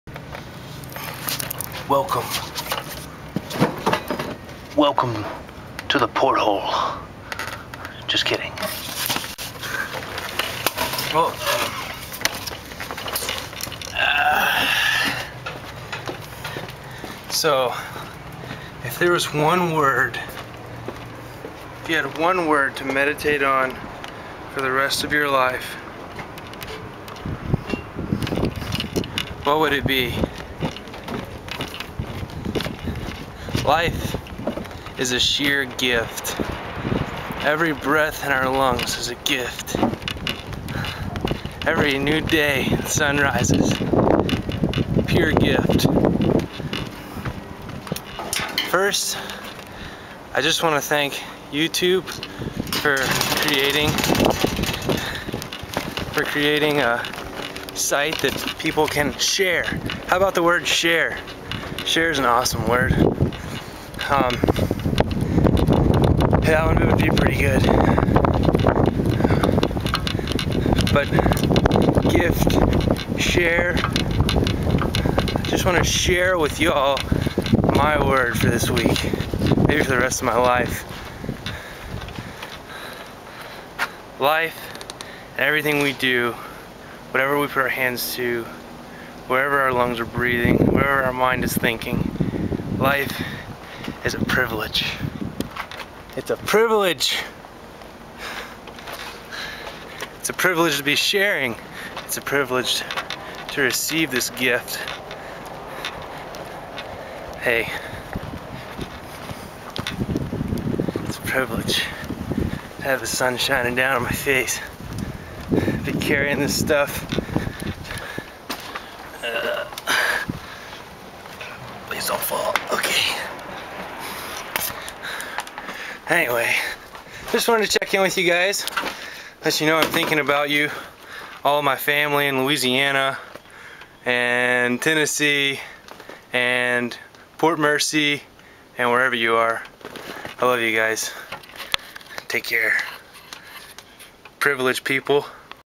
PRIVILEDGED.
regular work day (does that exists with friendships?). My A.D.D brain - as my mother renamed 'Adonai's Delighted Dude' or something like that - decided to put the camera on some luggage i was taking off of the pearl and loading in a trailor, and share my scattered rabbit-trail thoughts with the world.  Priviledged people we are.

p.s. - sorry if i don't make sense sometimes. These videos were originally intended for family only to view...
What i really mean to say is that i'm a 'rob bell failure'.

stay colorful.
gray